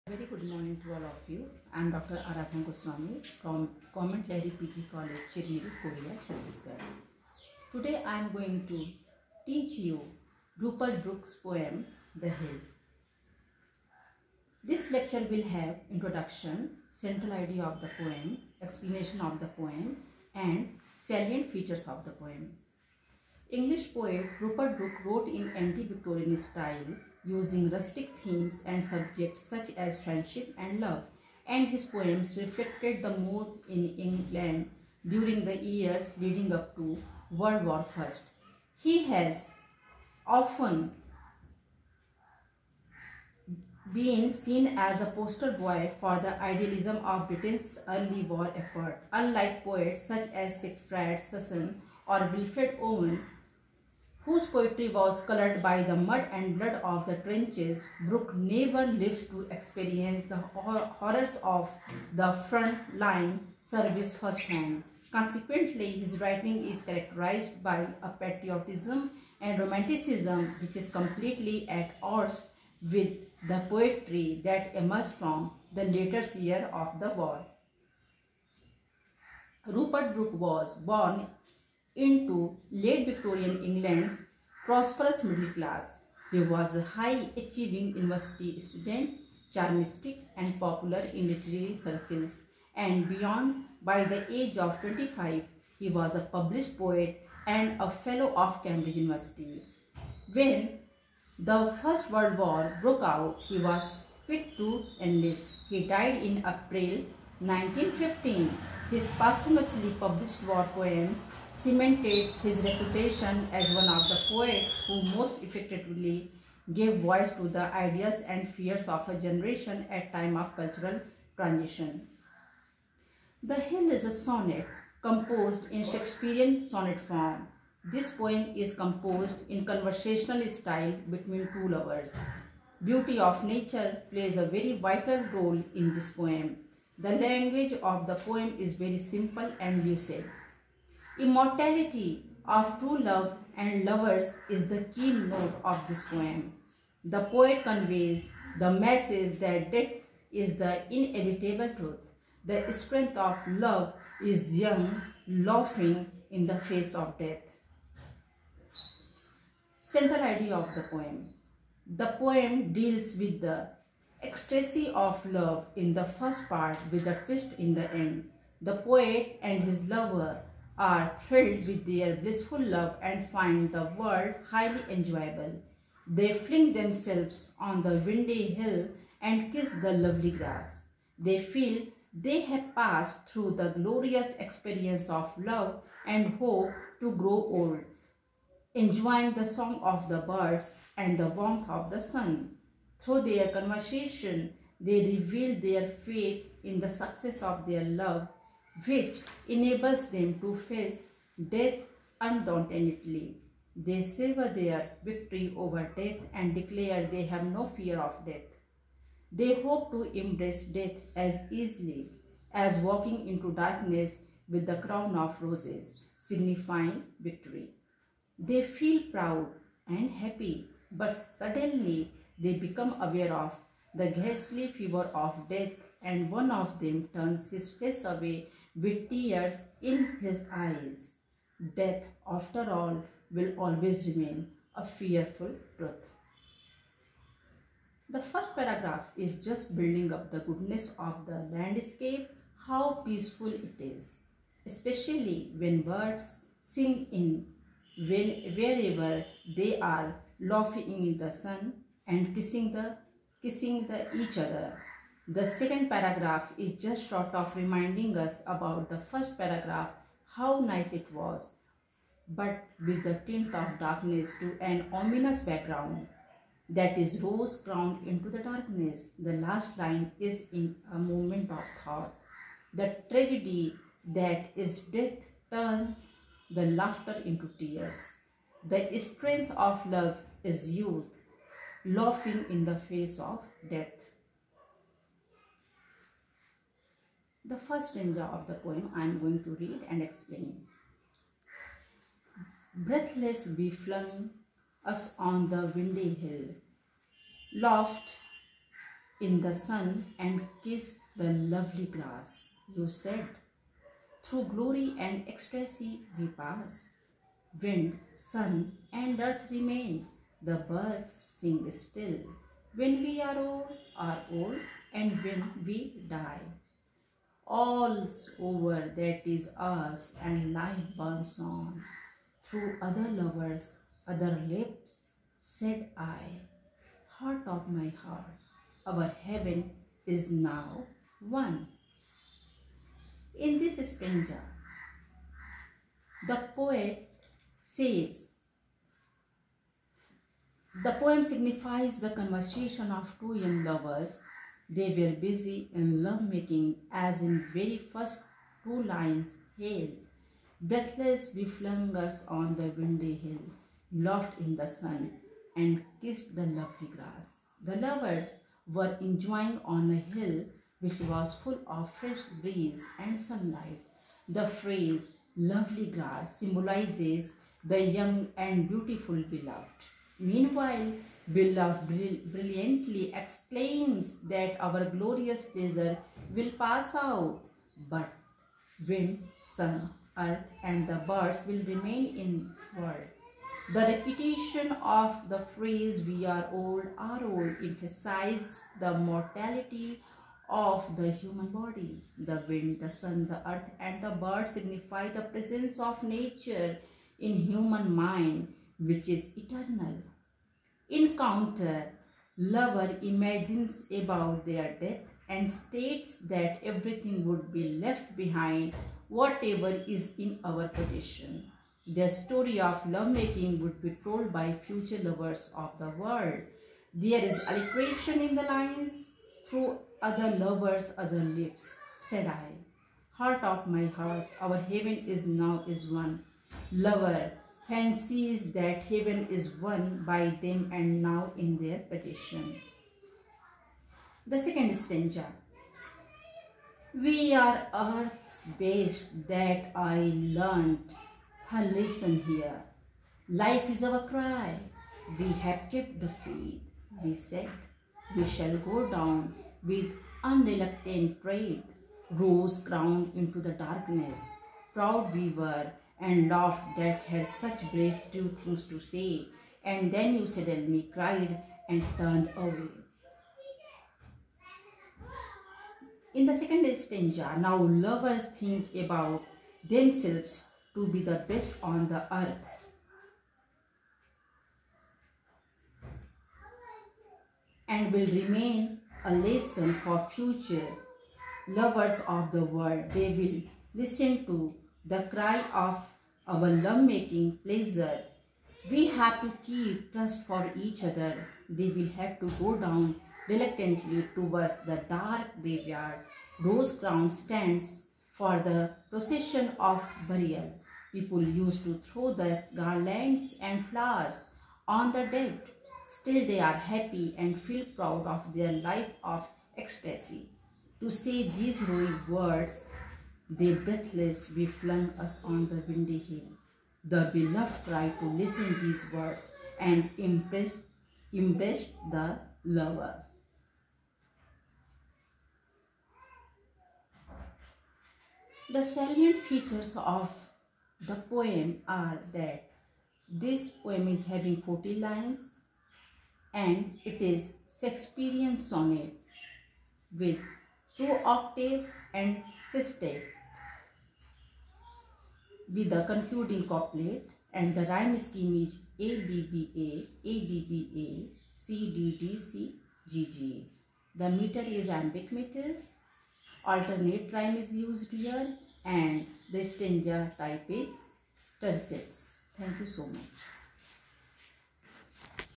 RUPERT BROOKE'S THE HILL- BA III ENGLISH LITERATURE
Themes and explanation of The Hill